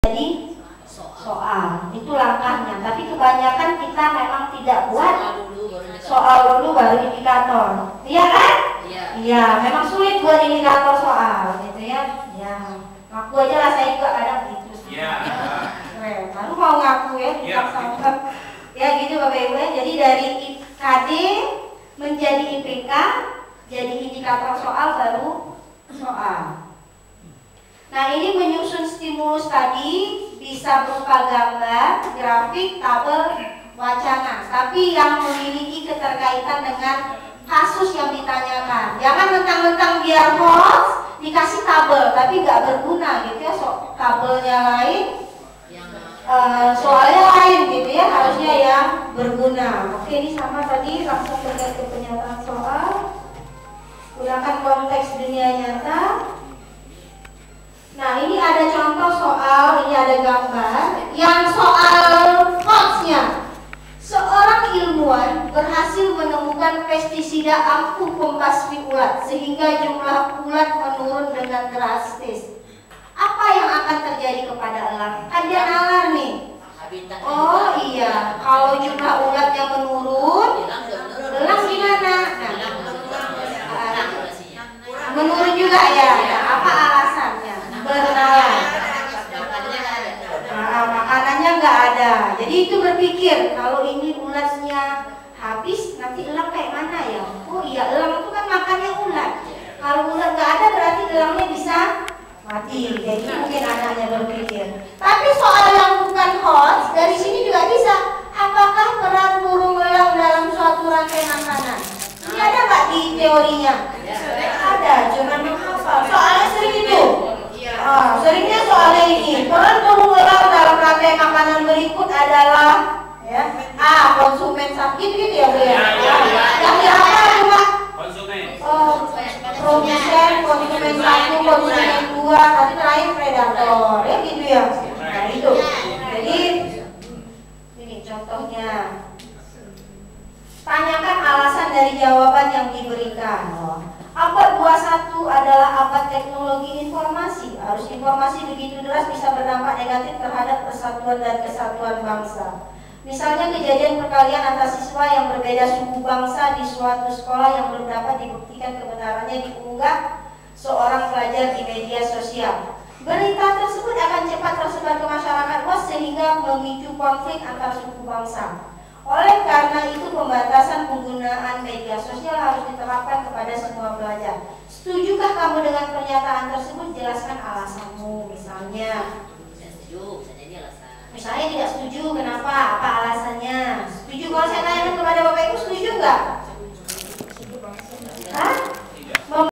MEMBUAT SOAL YANG BERKAITAN DENGAN PREDATOR, BY DUWI TERI
cara membuat soal, mulai dengan kisi-kisi. kata M.Rakib Jamari